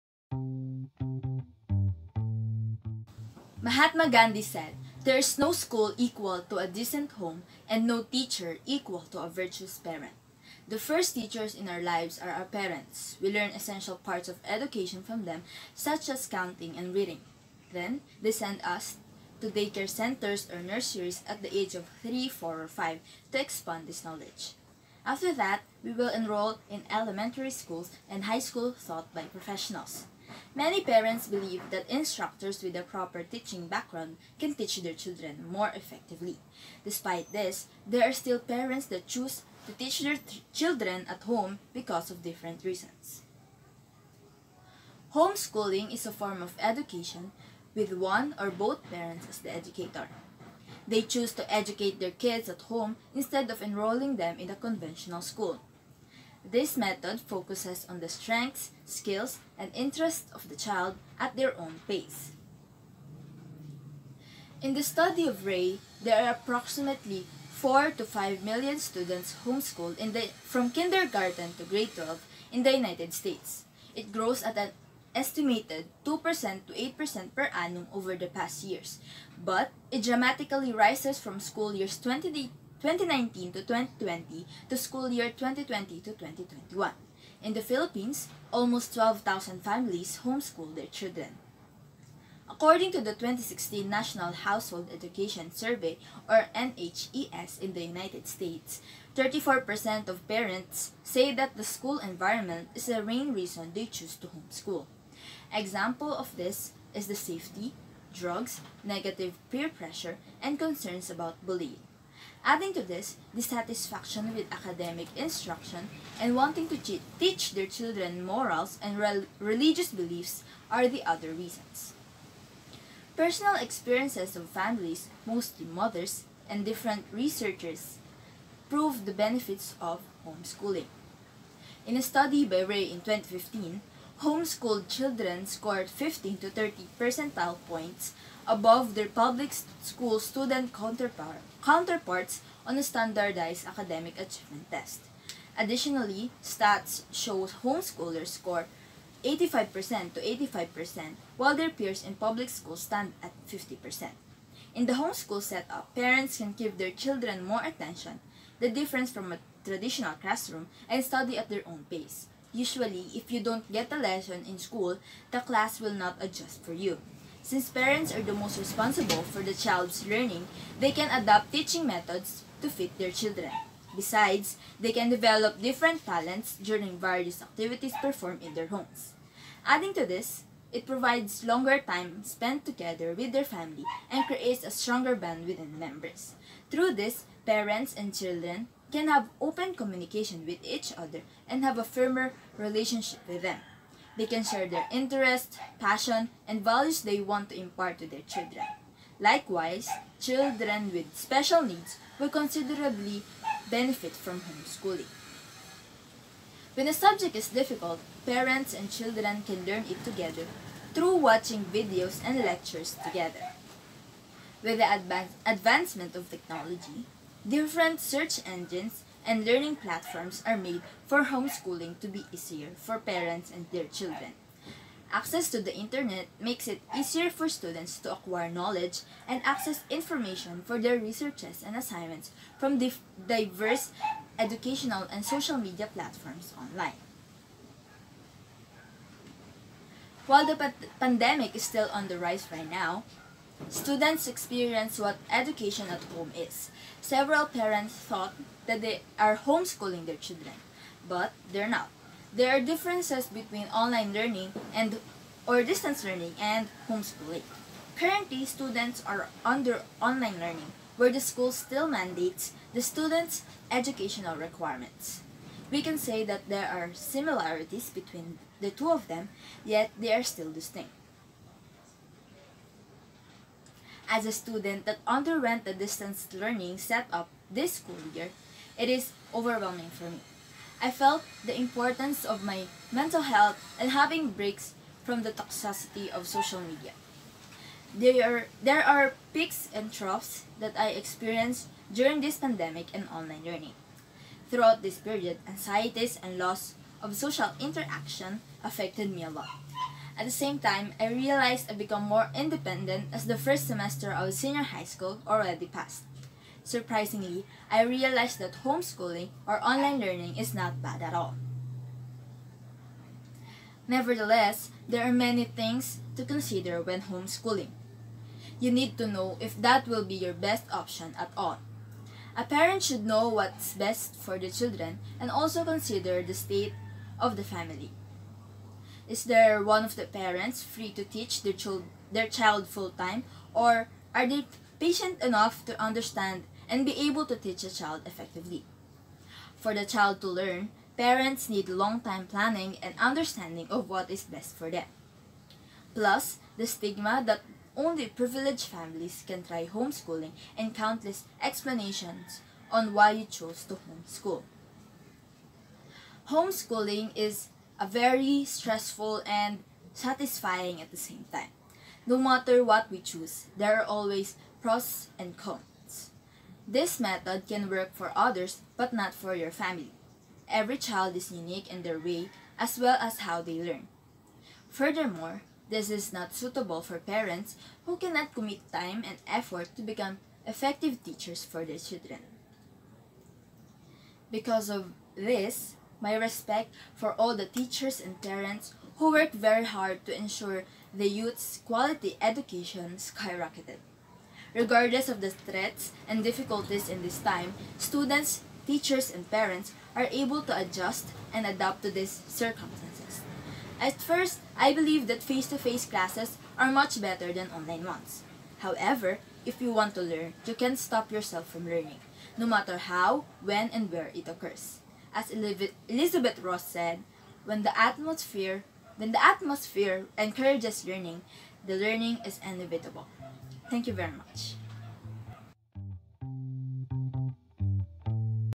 Oral Communication Output: Advantages of Home Schooling -Elyze Patricia Mastrili Why Home Schooling?
Indeed, it is true that If someone wants to learn, he/she can't be stopped fom learning.